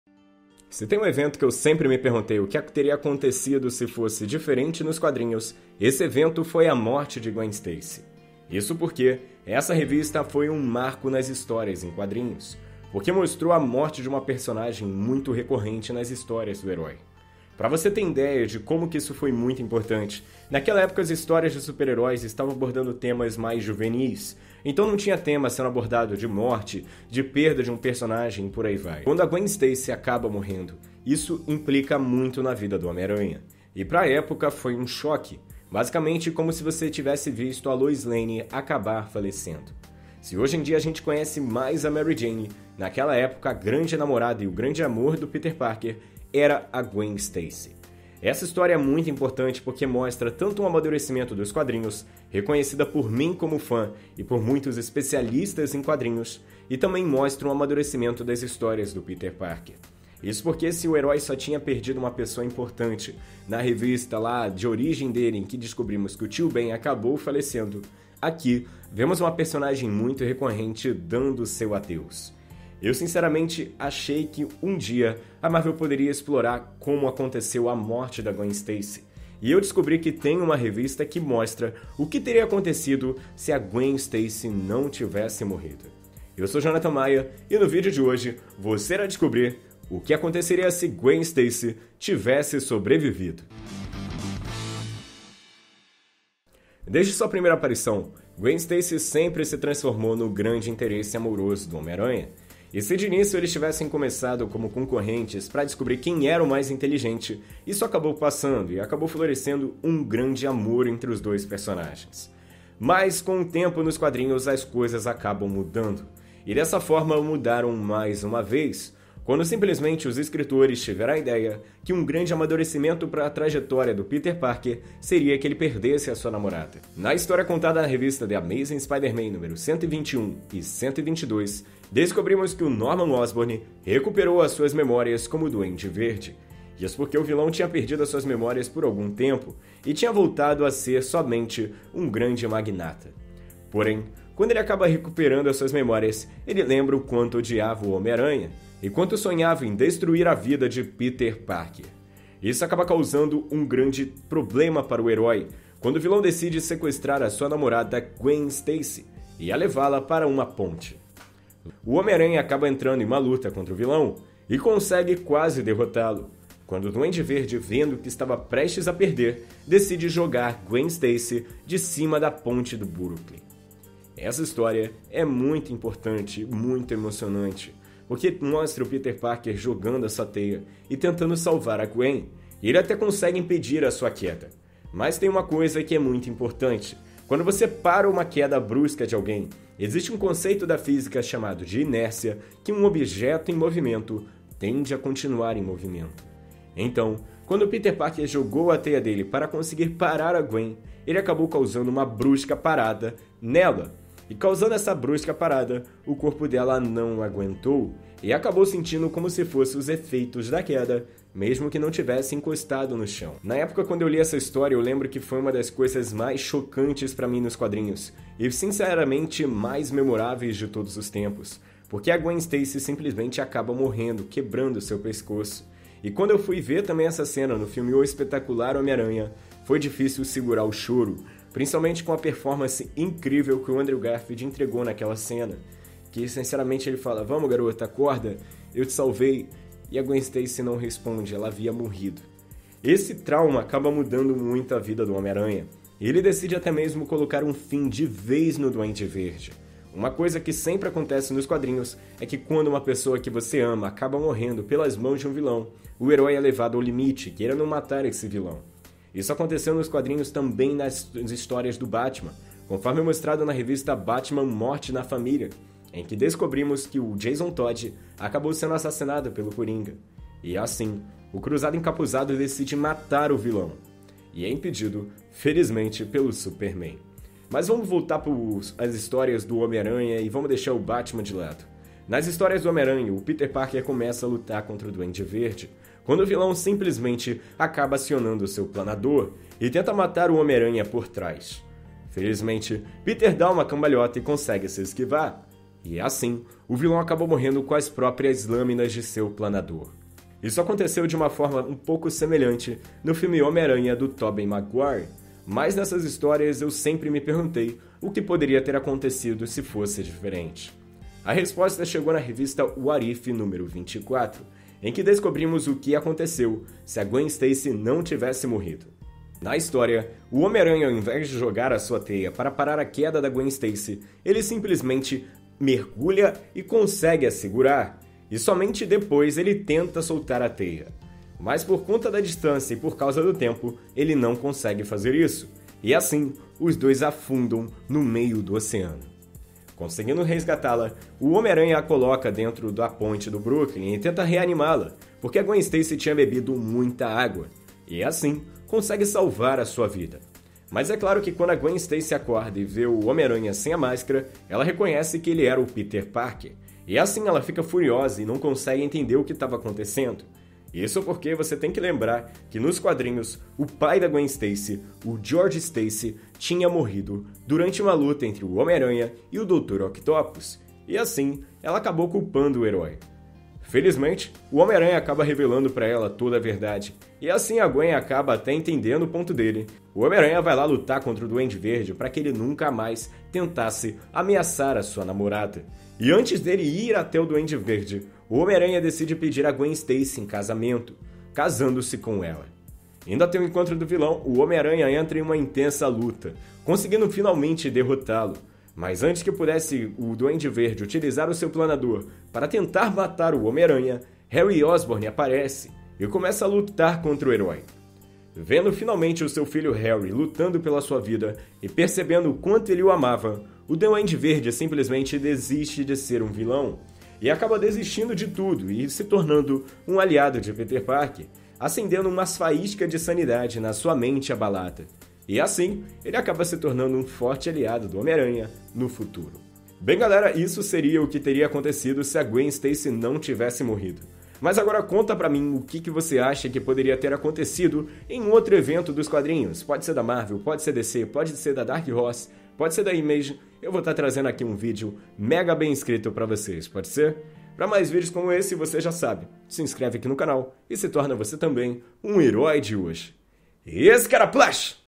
e se... GWEN STACY NÃO TIVESSE MORRIDO?
Alguns eventos nos quadrinhos impactam a vida dos super-heróis. No caso do Homem-Aranha, um dos eventos mais marcantes foi a morte de sua namorada Gwen Stacy pelas mãos de seu arqui-inimigo Duende Verde.
Nesse video, Jonathan Maia reimagina como seria se Gwen Stacy não tivesse falecido. contando a História da Revista What If #24, estreando o novo quadro no canal: o "O que aconteceria se"